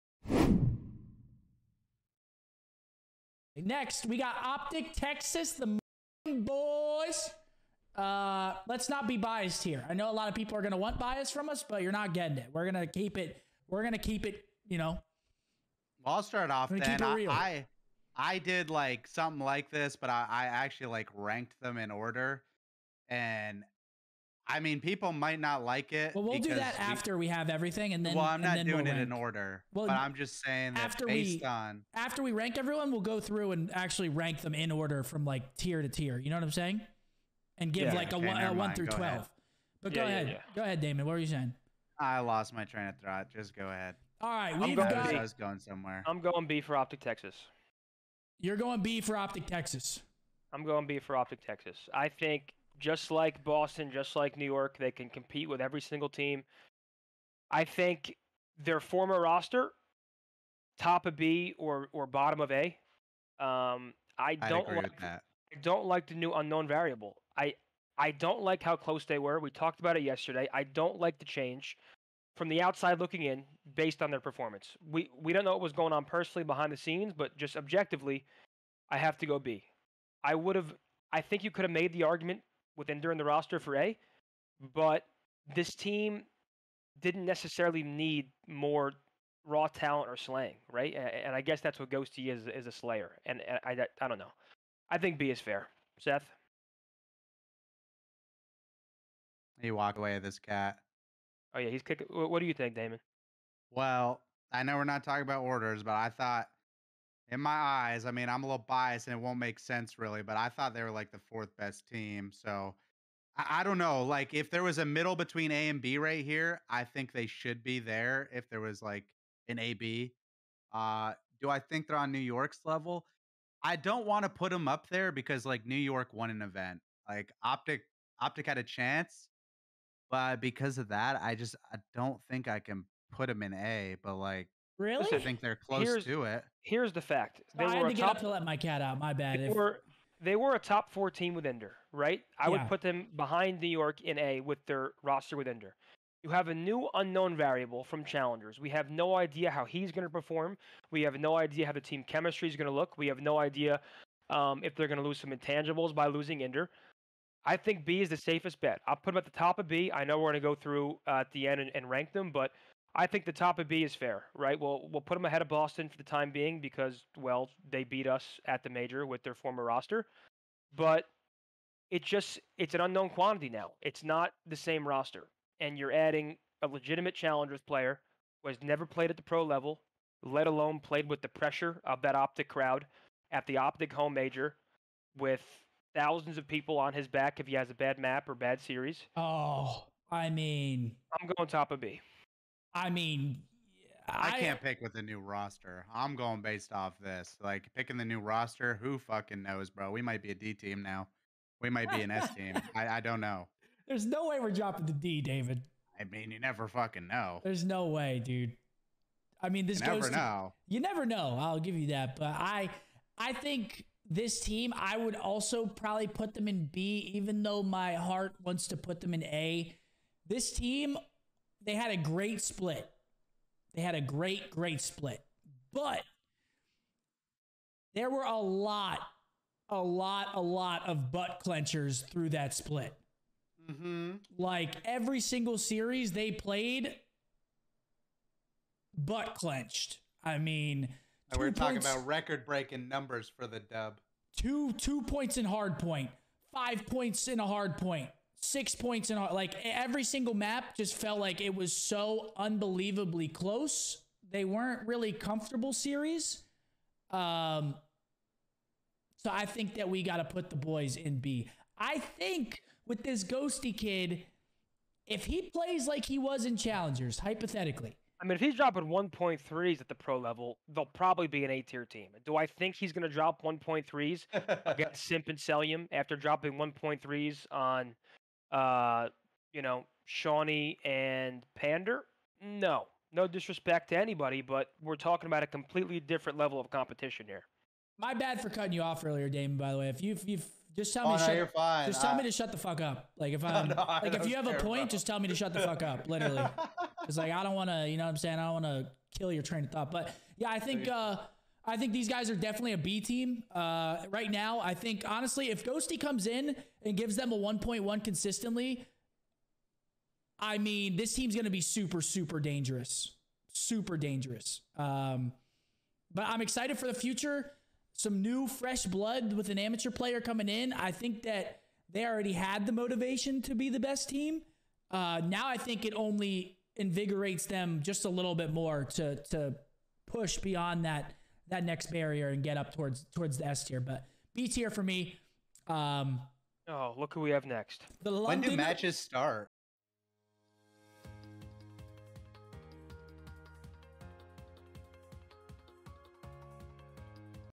Scump, Karma and Methodz on OpTic's current status
Hope everyone is having a great day, in this video Scump, Methodz, and Karma debate and rank OpTic's tier

If you want to be featured on the channel make sure to send your clips to and make sure to check out this great streamer